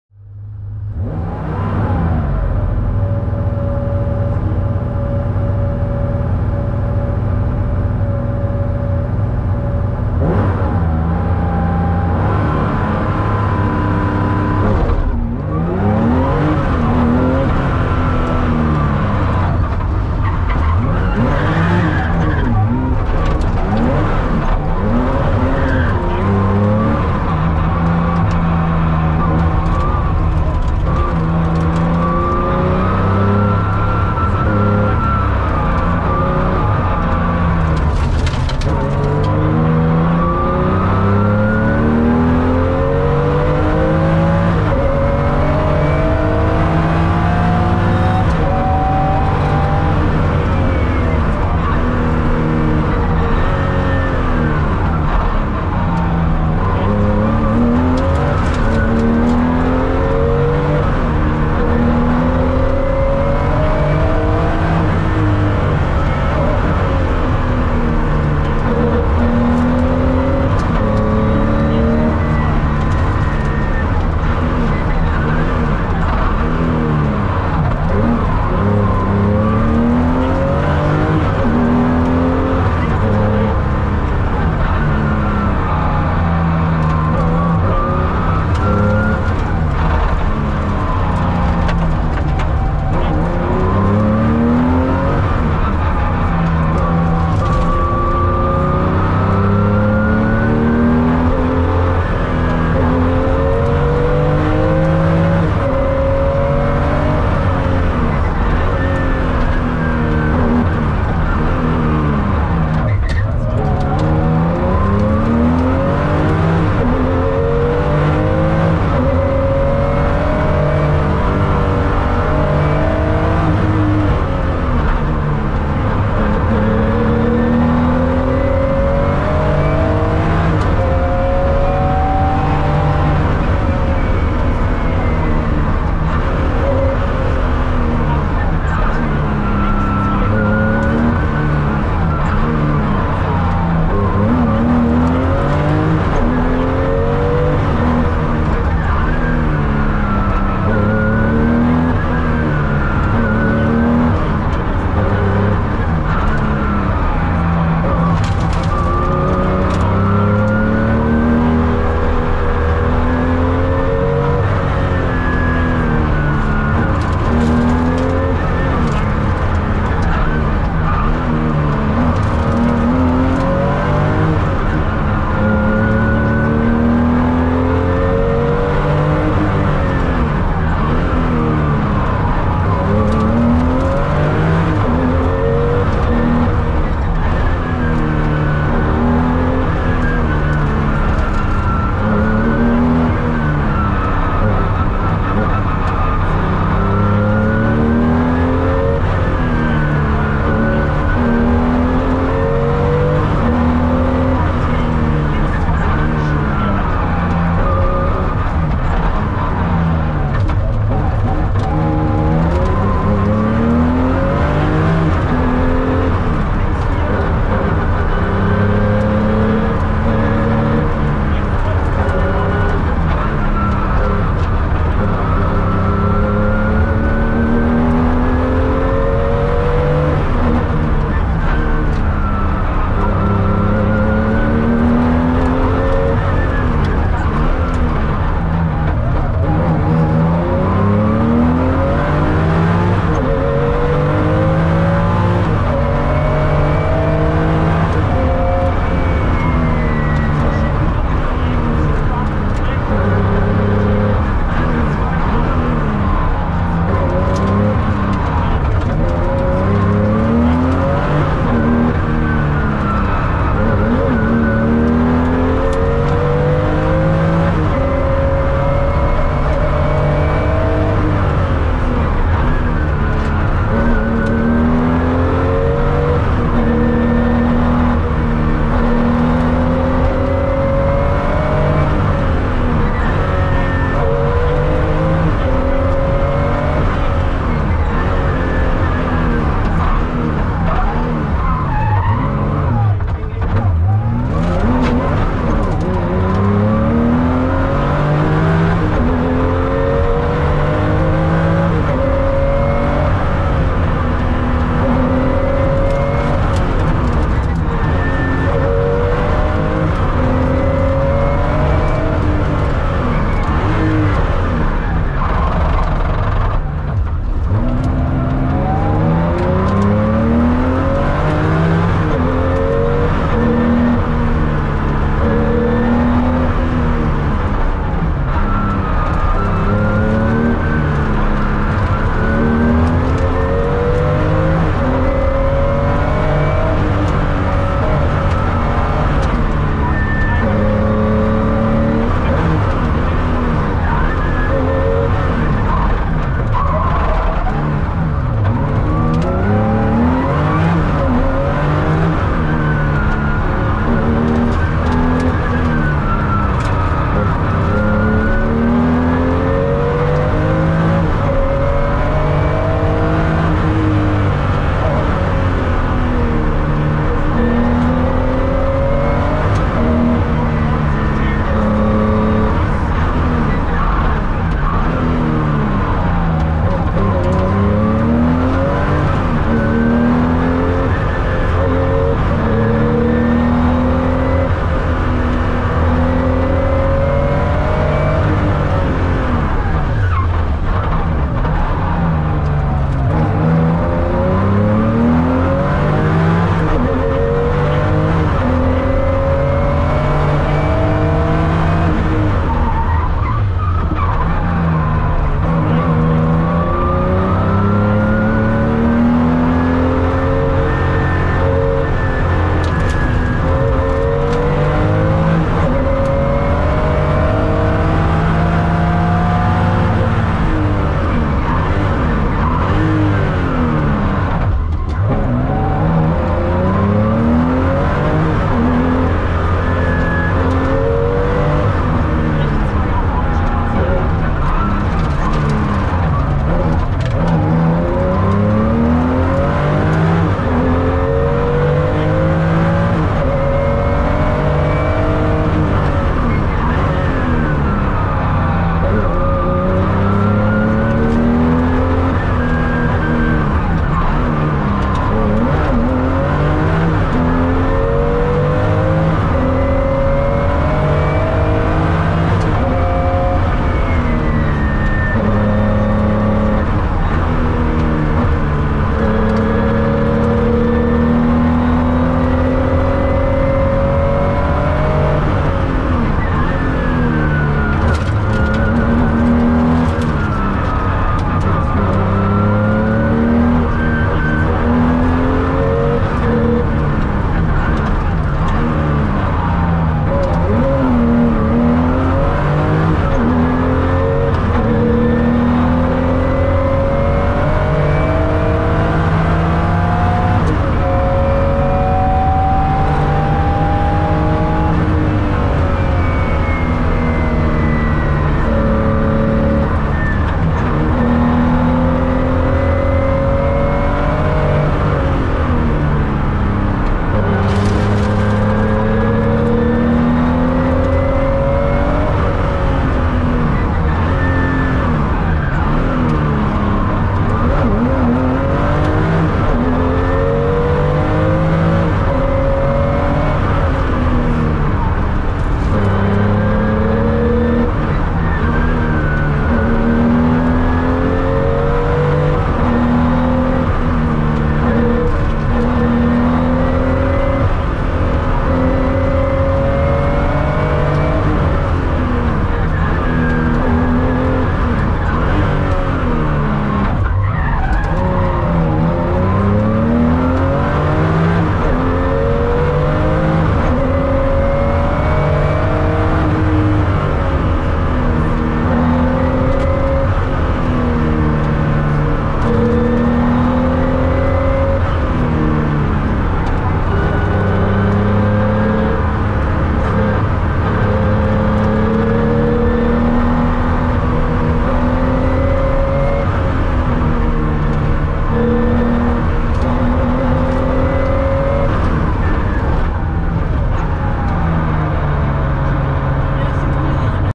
OnBoard - Porsche 911 Carrera - 3-Städte Rallye 2021- Klafferstraß - Andreas Dahms
OnBoard im Porsche 911 Carrera (1979) bei Andreas Dahms und Paul Schubert bei der 3-Städte Rallye 2021 auf der WP2 Klafferstraß I - 13.72 km